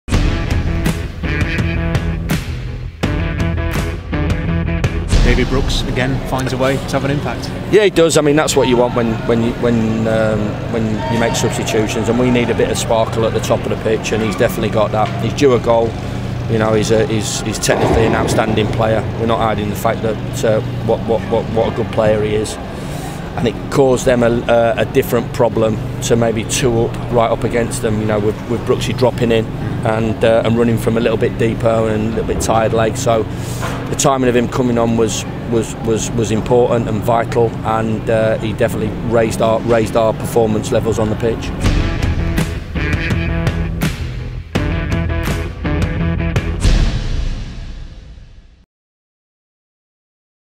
Chris Wilder on David Brooks winner
The Blades boss was delighted with David Brooks' late contribution at Elland Road - which lifted the Blades to the top of the Sky Bet Championship